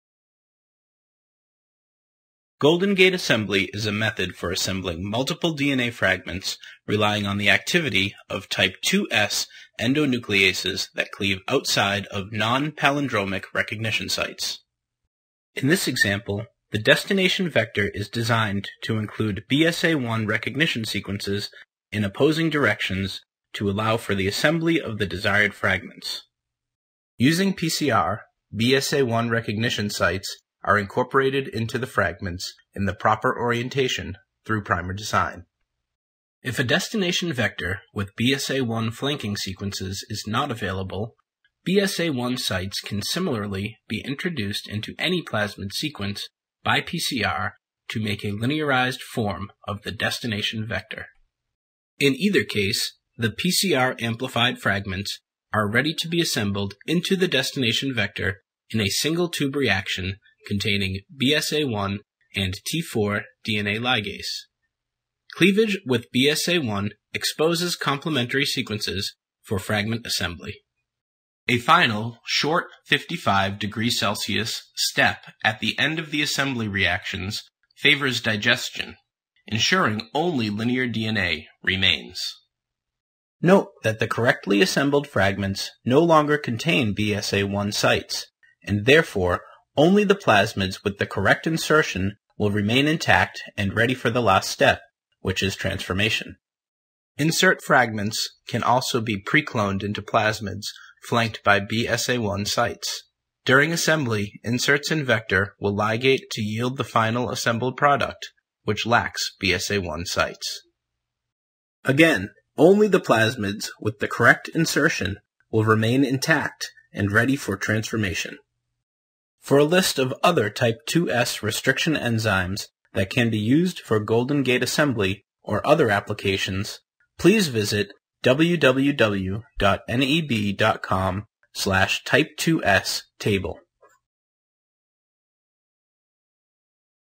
Golden Gate Assembly Workflow
Find out how Golden Gate Assembly can be used to quickly join multiple DNA fragments.